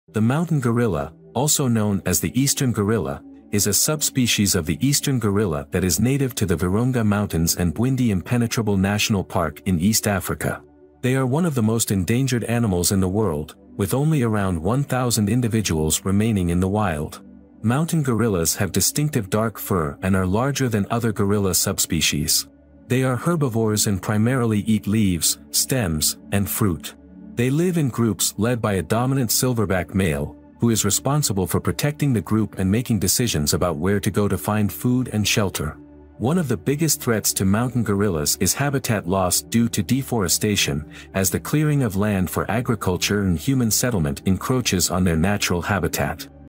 Mountain Gorilla: Endangered Animal!
Mountain gorillas are a subspecies of the eastern gorilla that lives in the forests of the Virunga Mountains and Bwindi Impenetrable National Park in Central Africa. They are the largest and most endangered of all gorillas, with only about 1,000 individuals left in the wild. They face many threats from habitat loss, poaching, disease, and human conflict. In this video, you will learn more about the biology, behavior, and conservation of these magnificent animals. You will also see some footage of them in their natural habitat, and how they live in social groups led by dominant silverback males. If you are interested in wildlife and nature, watch this video and discover the secrets of the mountain gorilla: endangered animal!